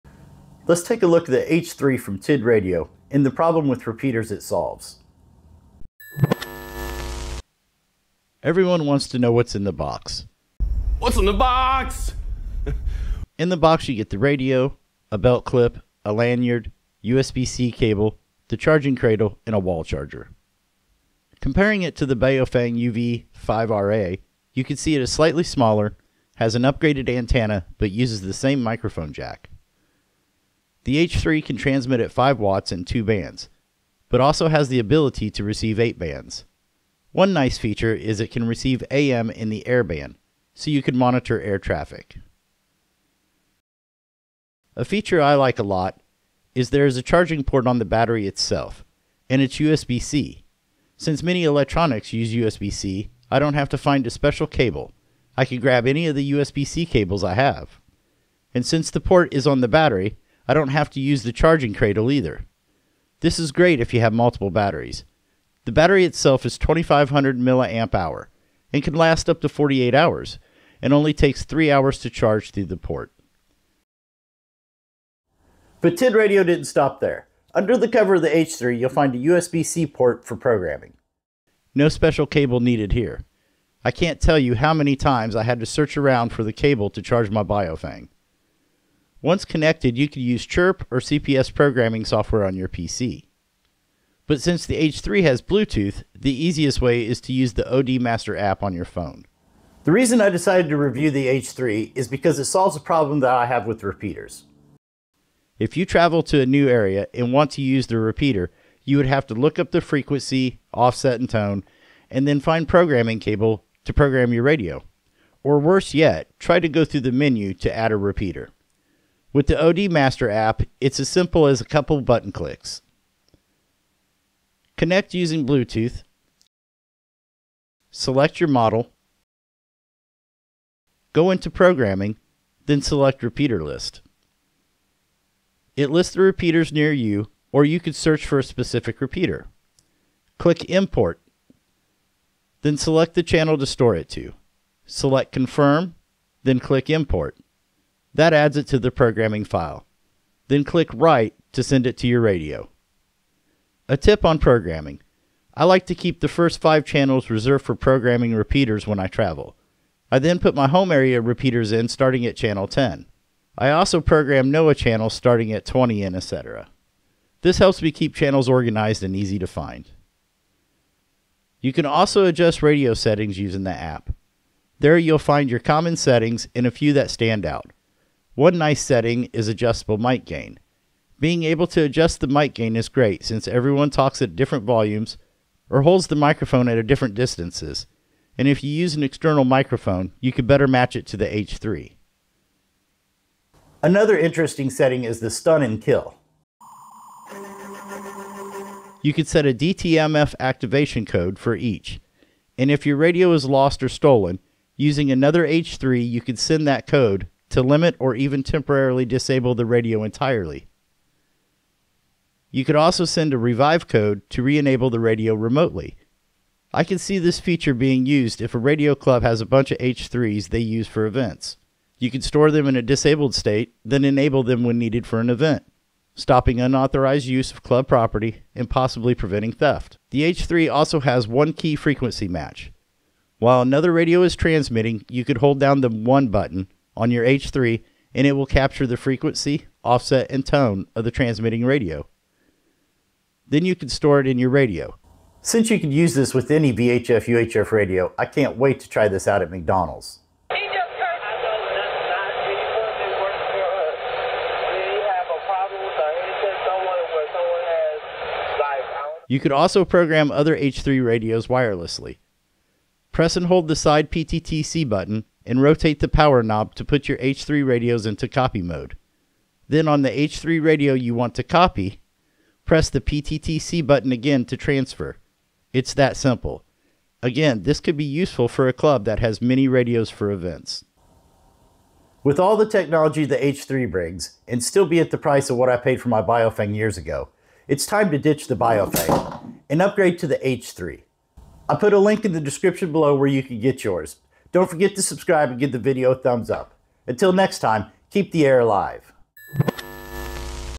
Why TIDRadio TD-H3 Beats Baofeng
The TIDRadio TD-H3 is here, and it’s solving some big problems for radio enthusiasts! In this video, I unbox the H3 and compare it to the popular Baofeng UV-5RA. Spoiler alert: the H3 is smaller, packs more features, and even includes USB-C charging.

The H3 is a dual-band radio (VHF/UHF) with 5W transmit power, plus the ability to receive across 8 bands, including AM Air Band for monitoring air traffic. It’s equipped with a 2500 mAh battery that charges via USB-C directly on the battery or through the cradle. No more hunting for special cables!

Programming is also a breeze. Thanks to built-in Bluetooth, you can use the ODmaster app to quickly add repeaters with just a few clicks. Whether you’re traveling or organizing your channel list, the app makes life simple. Plus, the USB-C programming port means no more special cable hassles.

The TD-H3 is packed with handy features:

Adjustable Mic Gain for crystal-clear audio.
Stun/Kill/Revive Functions to protect your radio.
One Key Frequency Match for copying settings from other radios instantly.
Wireless Cloning between H3 units for group use.
This radio is a game-changer, especially at this price point. Whether you’re looking to upgrade from your Baofeng or need a powerful, feature-rich radio for your adventures, the H3 has you covered.

Chapters:
0:00 - Intro
0:10 - Unboxing the H3
0:20 - Comparison to Baofeng UV-5RA
0:34 - Bands, Features, and Air Band Reception
0:44 - USB-C Charging and Battery Life
1:38 - Programming with Bluetooth and ODmaster App
2:32 - Tip for Channel Organization
2:50 - Standout Settings: Mic Gain and Stun/Kill Functions
3:50 - One Key Frequency Match and Wireless Cloning
4:40 - Why It’s Time to Ditch the Baofeng

Until next time, tune in, geek out, and Keep the Air Alive!